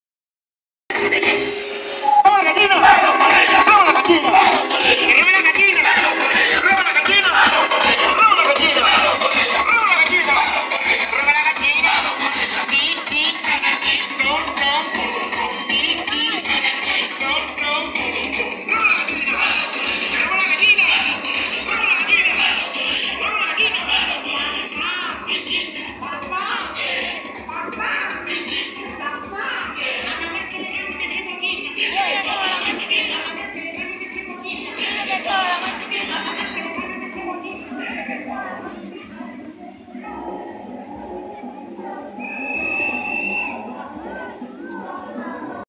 Robalagallina danzantes. Cierre de Carnaval Santo Domingo 2023.
Engalanadas robalagallina, baile y paraguas.